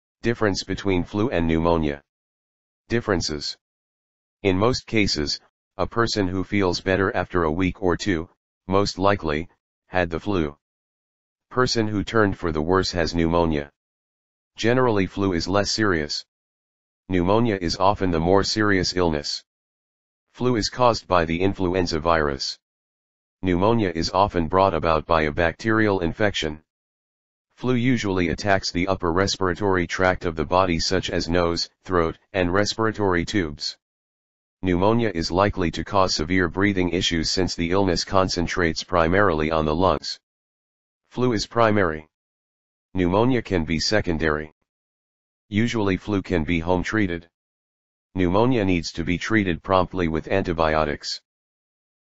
Differences of Flu and Pneumonia
As the names suggest, bacteria cause bacterial infections, and viruses cause viral infections. ... Examples of bacterial infections include whooping cough, strep throat, ear infection and urinary tract infection (UTI). Viral infections include the common cold, flu, most coughs and bronchitis, chickenpox and HIV/AIDS.Have you ever had a bad cold or a nagging cough and wondered if it was a virus or bacterial infection? ... The symptoms of a bacterial infection and a virus are often very similar—fever, muscle aches, cough, and sore throat—but they require different treatments.Even without antibiotics, most people can fight off a bacterial infection, especially if symptoms are mild. About 70 percent of the time, symptoms of acute bacterial sinus infections go away within two weeks without antibiotics.Your doctor may prescribe antibiotics if the symptoms are severe and include high fever along with nasal drainage and a productive cough. Antibiotics may also be necessary if you feel better after a few days and then your symptoms return or if the infection lasts more than a week.Antibiotics are the recommended treatment for bacterial vaginosis. A few antibiotic remedies are routinely used and include: metronidazole (Flagyl) taken by either oral (pill) form or vaginal metronidazole gel (Metrogel).Viral vs. Bacterial Pneumonia. Different causes will produce different symptoms of pneumonia. Viral pneumonia usually causes milder symptoms, such as a dry cough and light chest congestion. ... When pneumonia develops as a complication of flu, it is more likely to be bacterial rather than viral pneumonia.People with bacterial pneumonia will often have colored mucus, a fever and chest pain. They should seek medical attention to prevent lung damage. Bacterial pneumonia can sometimes develop after a cold or flu and often is caused by exposure to the streptococcus pneumonia germ.